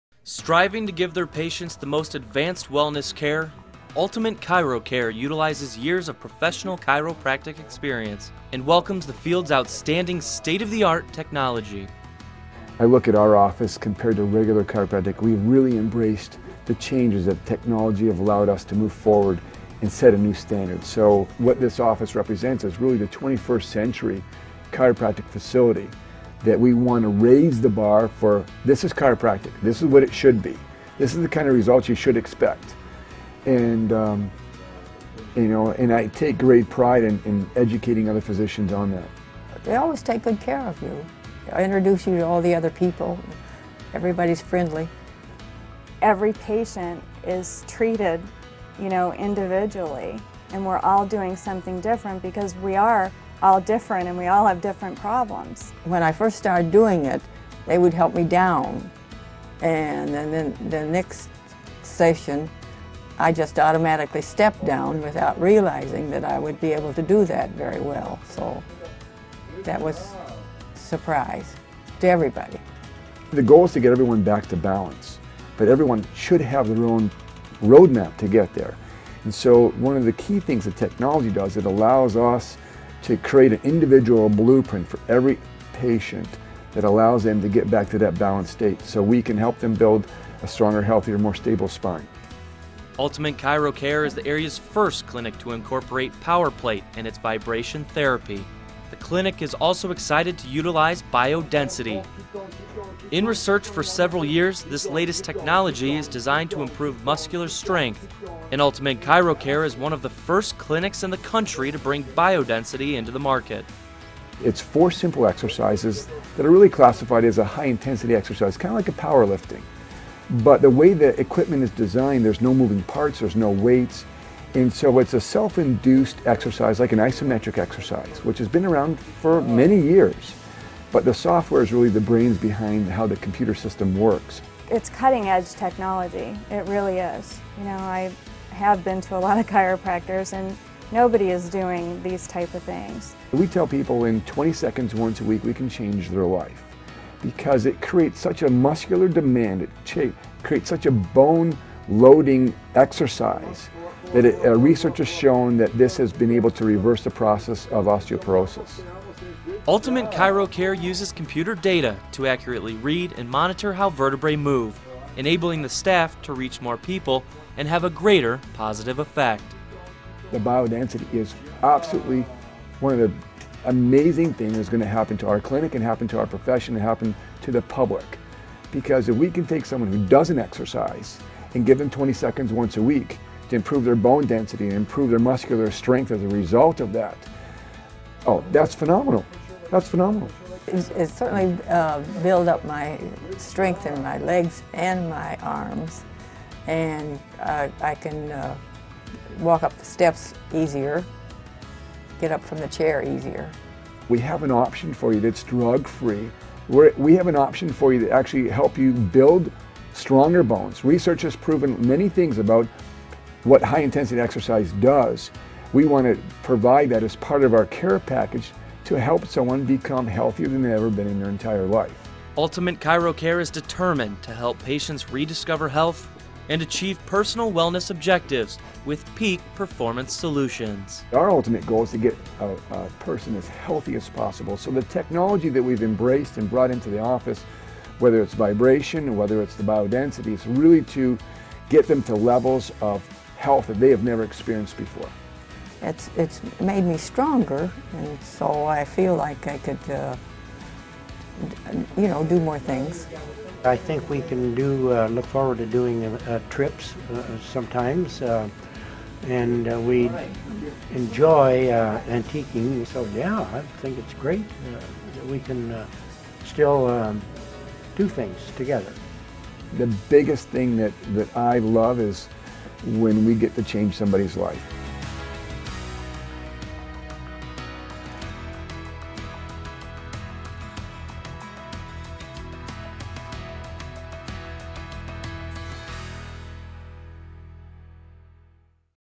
Dr. Cammisa of Ultimate Chiro Care
Dr Cammisa of Ultimate ChiroCare introduces BioDensity as a part of the regimen he uses in his clinic